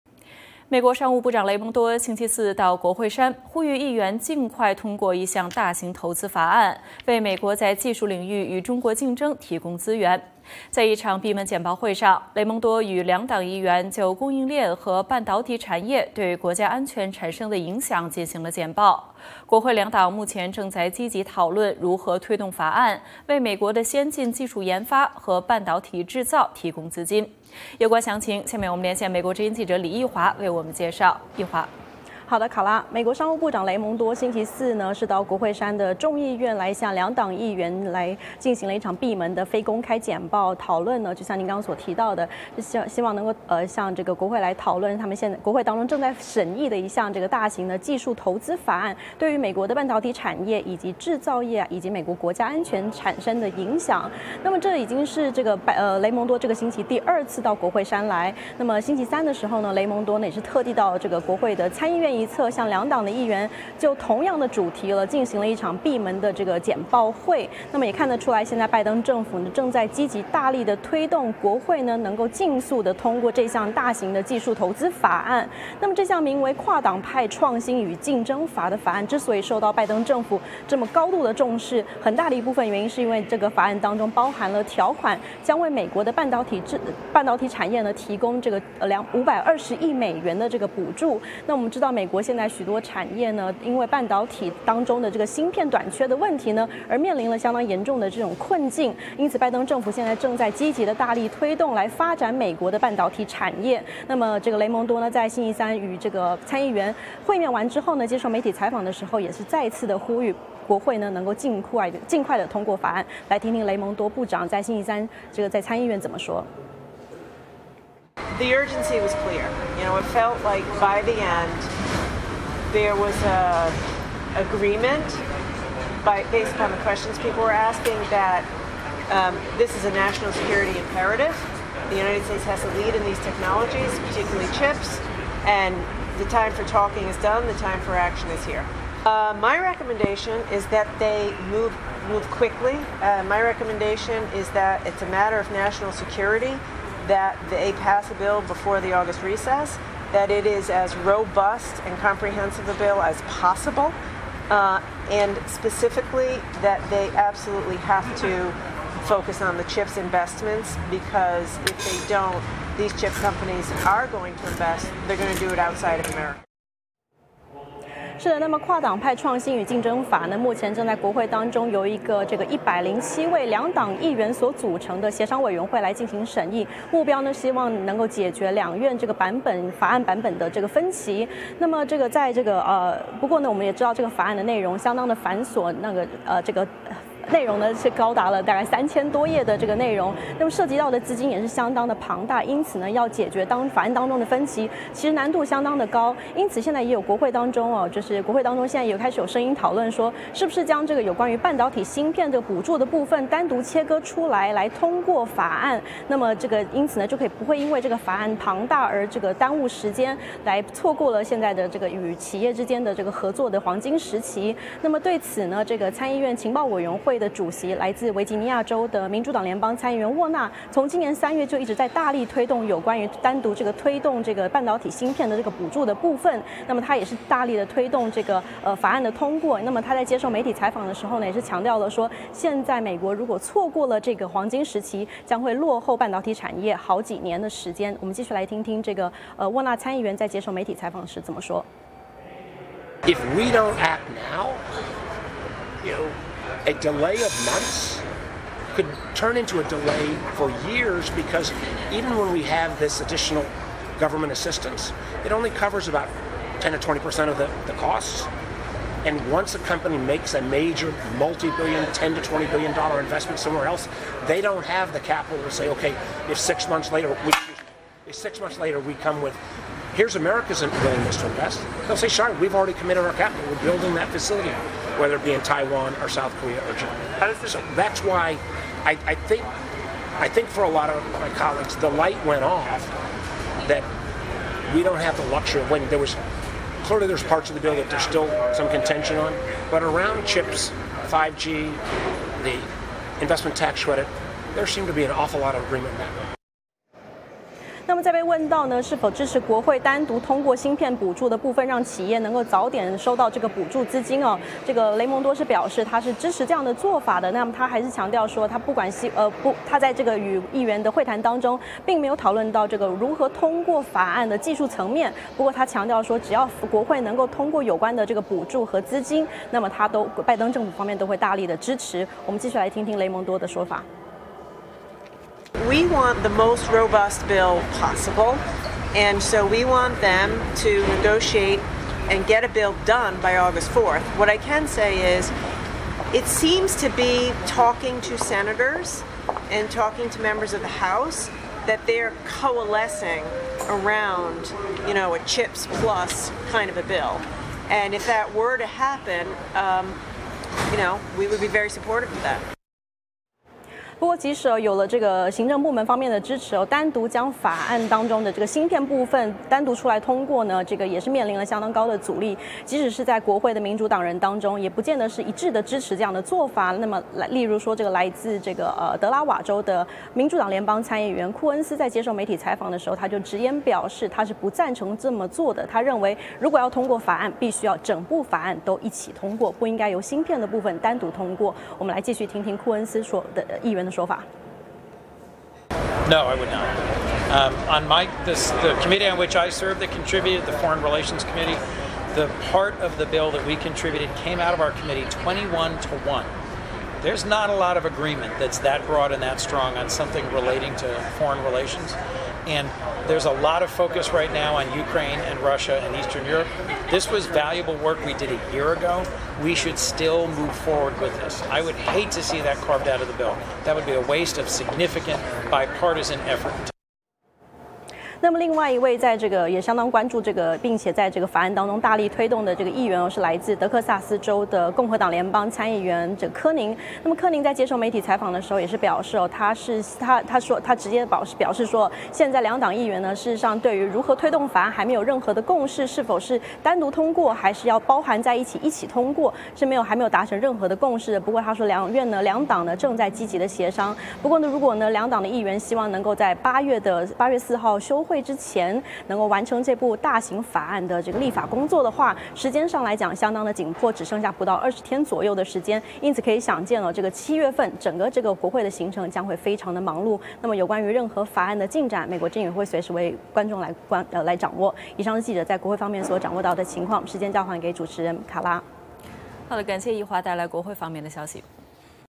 美商务部长本周二度赴国会 敦促尽快通过抗中法案
美国商务部长雷蒙多星期四到国会山，呼吁议员尽快通过一项大型投资法案，为美中在技术领域的竞争提供资源。在一场闭门的简报会上，雷蒙多与两党议员就供应链和半导体产业对国家安全产生的影响进行了简报。国会民主、共和两党目前正积极讨论如何推动法案，为美国先进技术研发和半导体制造提供资金。